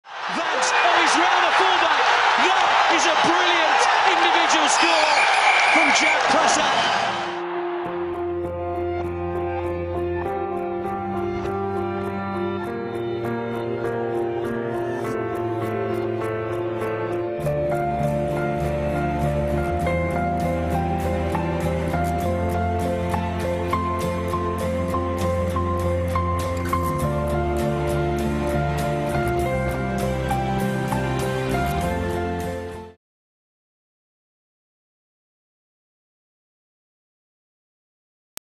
ARU - History begins again Prasad
Created By J. Fowke

A short promo for the Army Rugby Union featuring Fly Half Jack Prasad - History Begins Again. Inspired by the NHL campaign.